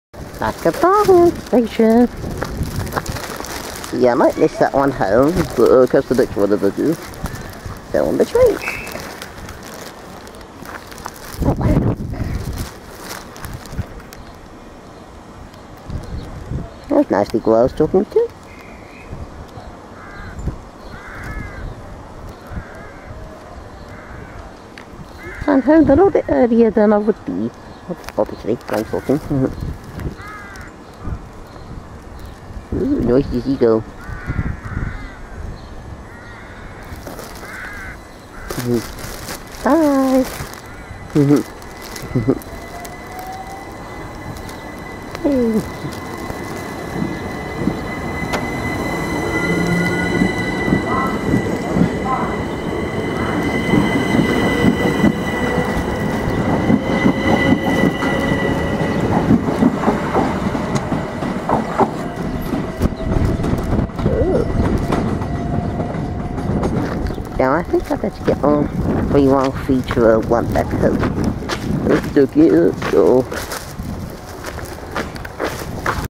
377-113 departs Barnham Station
I film the train i was on leave then wait for my one, 4th May 2013.